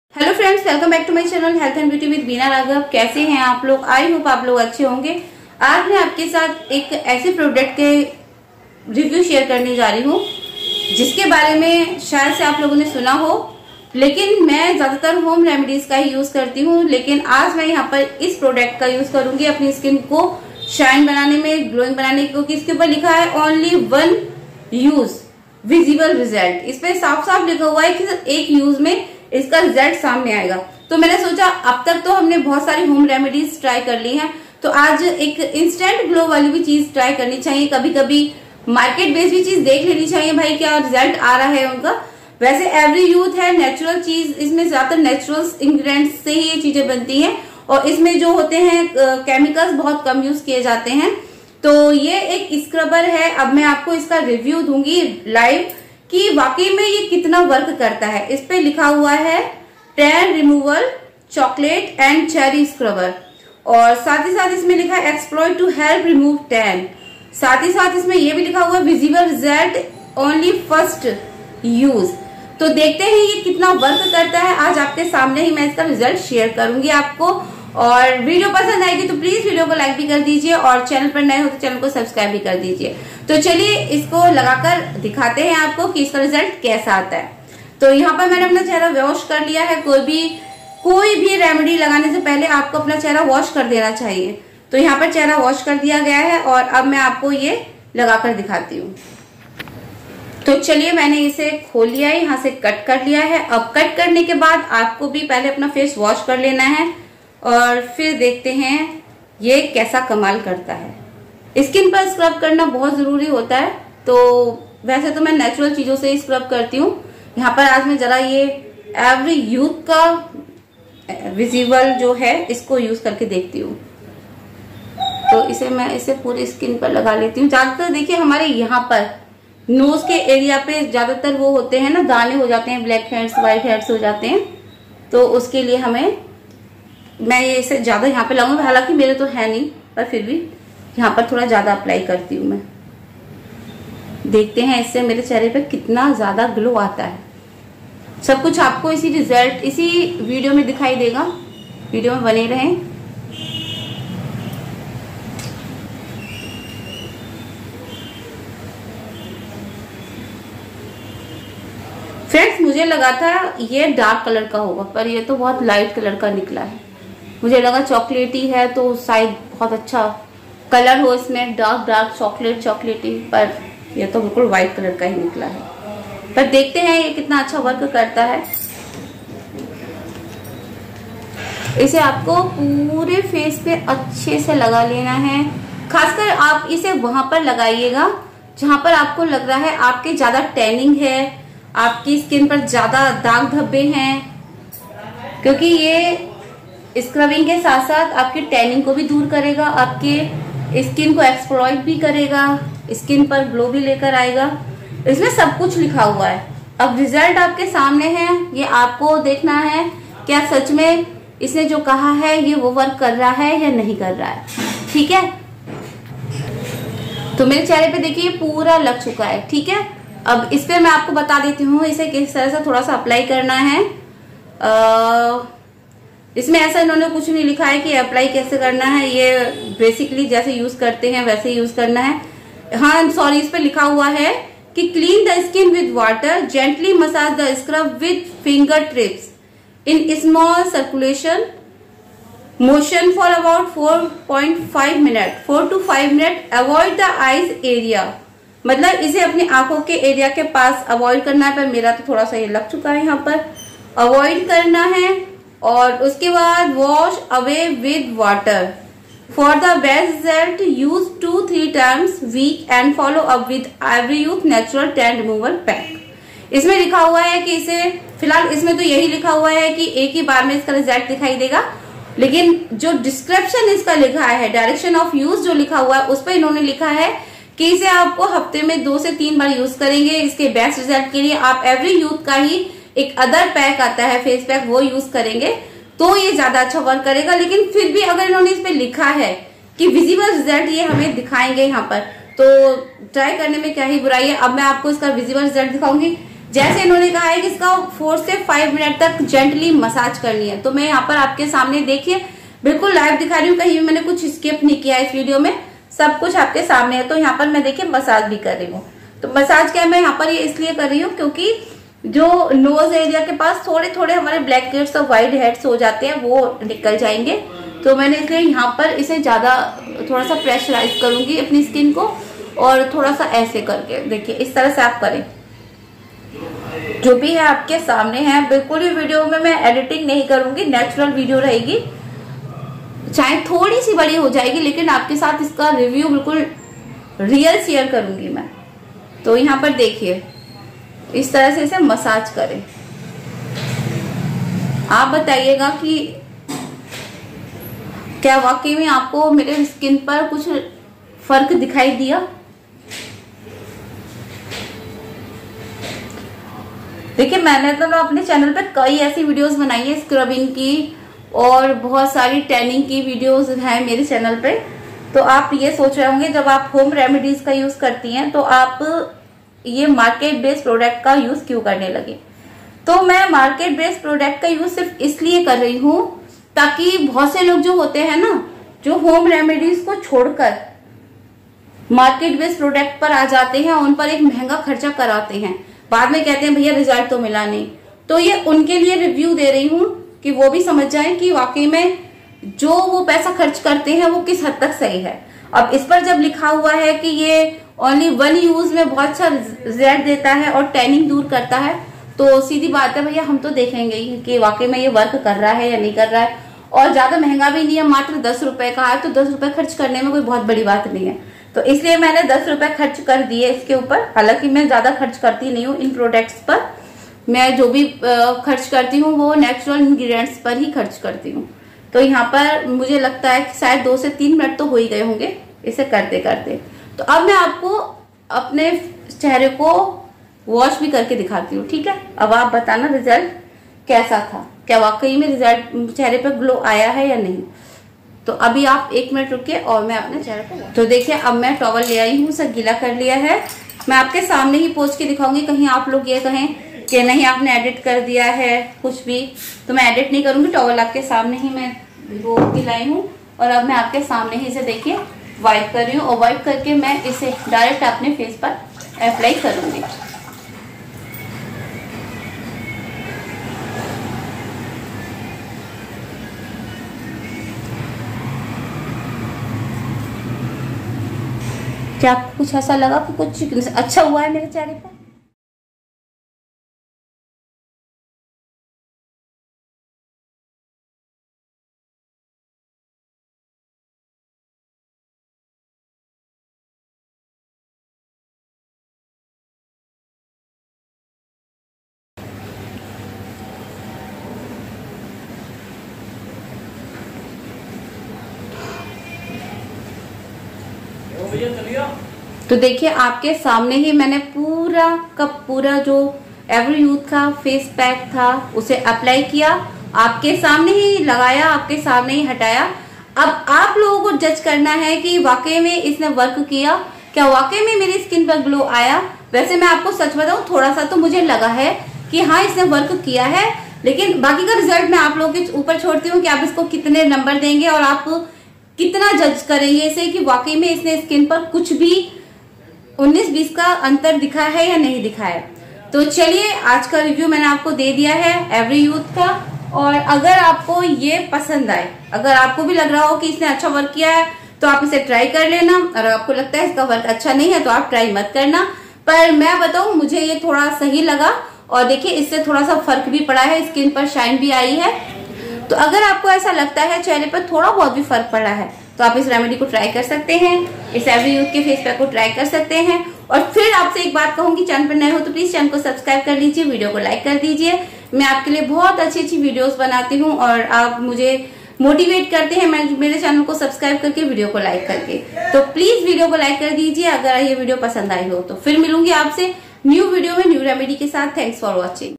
Epic Scrubber Removes All my impurities | HONEST REVIEW Of Tan Removal scrub
Epic Scrubber Removes All my impurities
HONEST REVIEW Of Everyuth Tan Removal scrub#review tanremovalscrub ‎@HealthAndBeauty-Veena-Raghav

Watch as I put an epic youth tennning removal scrubber to the test in this satisfying video! See how this scrubber effortlessly removes all impurities from my skin, leaving it feeling refreshed and rejuvenated. Say goodbye to clogged pores and hello to a cleaner, clearer complexion with this amazing scrubber. Don't miss out on witnessing the transformation for yourself!